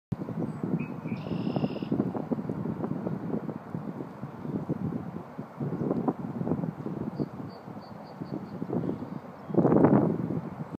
Red Ant hill
Here is an ant-hill built by red-ants.